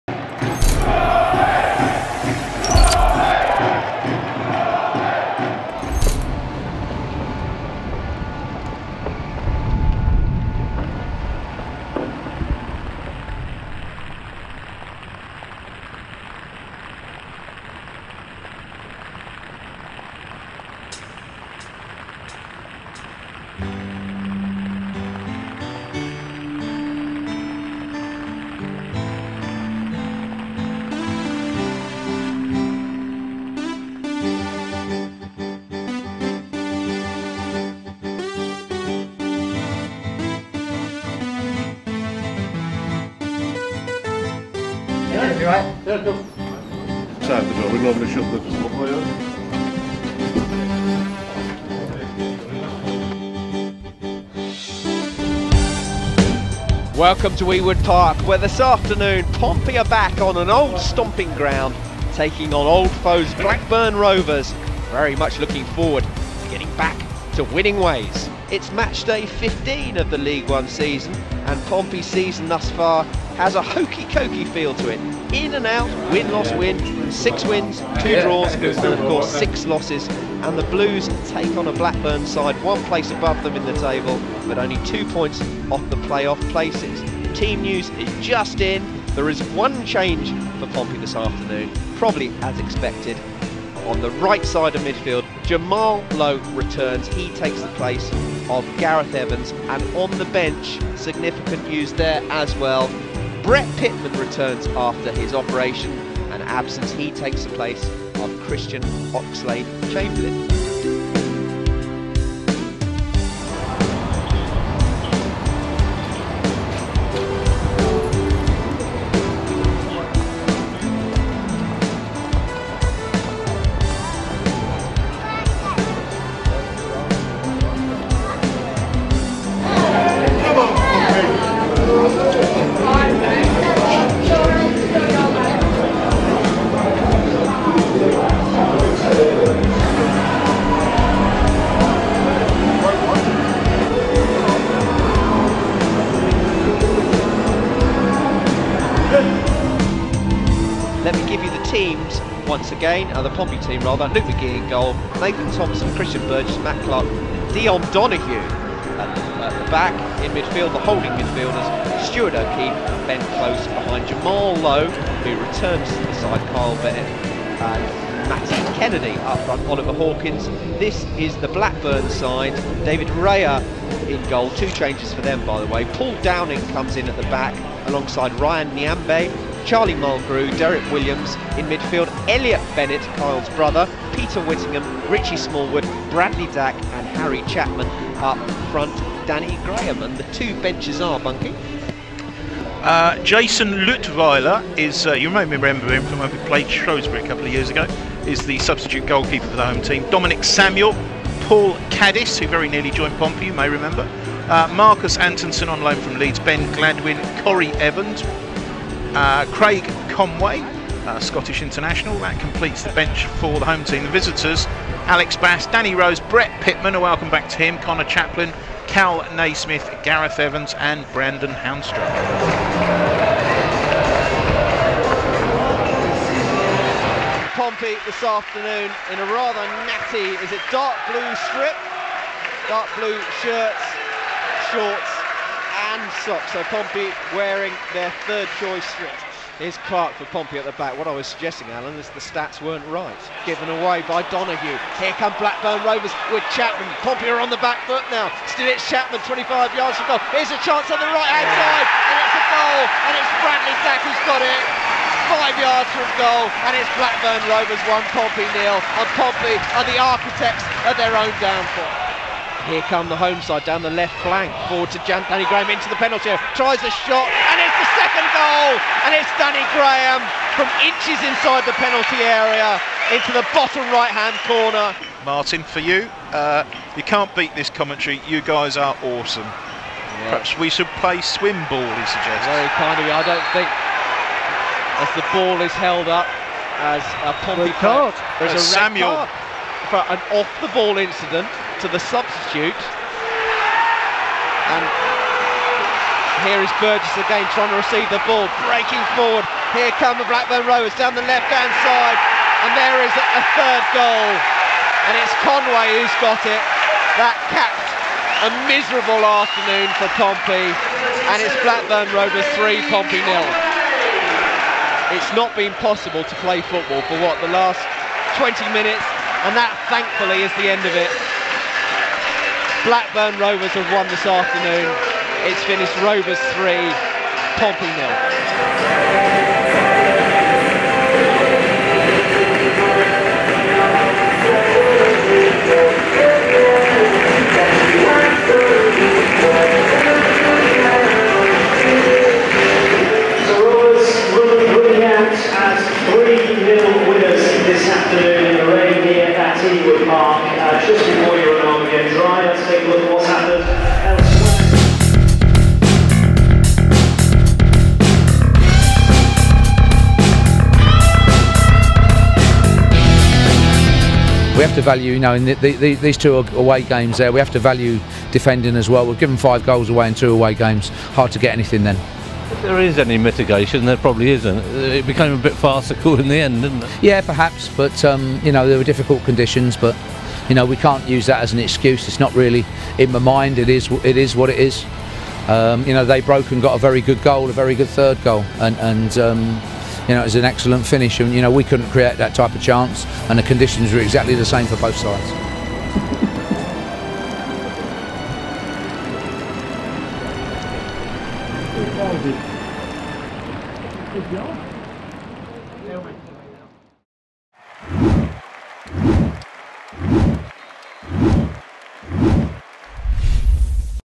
Inside Out: Blackburn Rovers
A behind-the-scenes look at Pompey's Sky Bet League One visit to Blackburn Rovers.

The Blues slipped to a 3-0 defeat at Ewood Park, in testing conditions, as their poor recent away form continued.